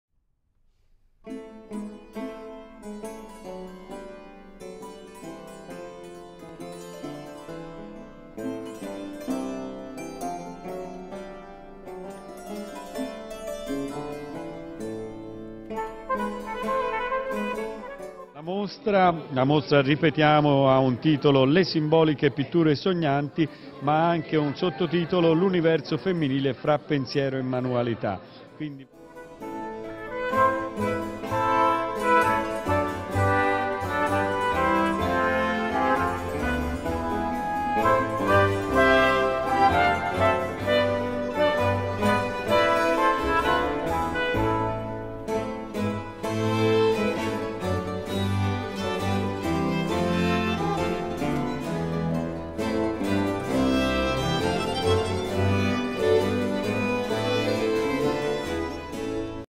Erni Kwast - Santo Stefano, Florence
Impressie van de uitzending op Toscana TV over de expositie van Erni Kwast in het Museo Diocesano di Santo Stefano, in Florence, 2008.
Interview: Fabrizio Borghini
Kunstcriticus: Giampaolo Trotta
Samenstelling: G.J. Venema